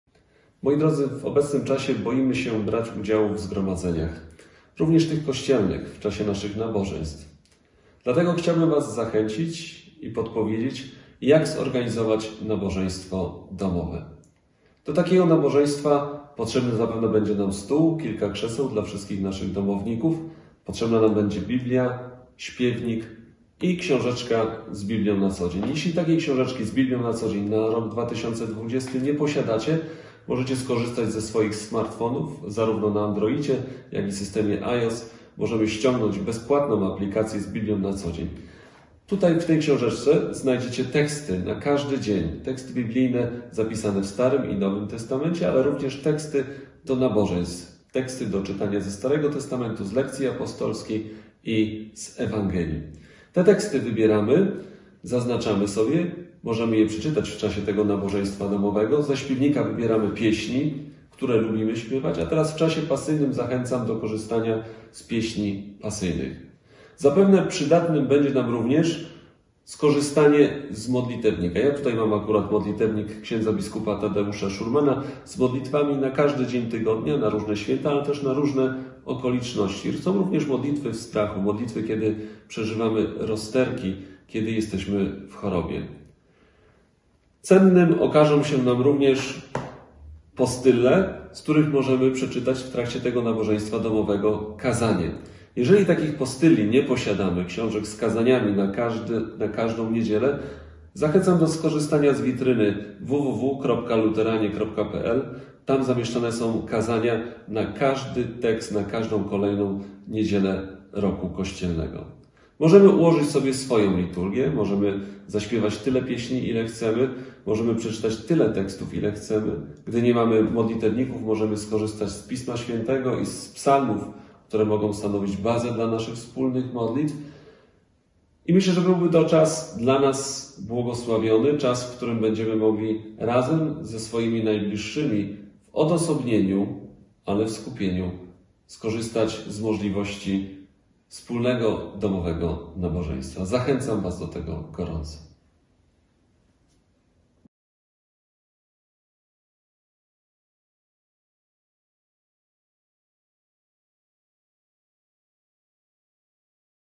Nabożeństwo domowe
Zapraszamy do organizacji nabożeństw domowych, gdy zalecane jest unikanie większych skupisk ludzi. Materiały do prowadzenia nabożeństw tutaj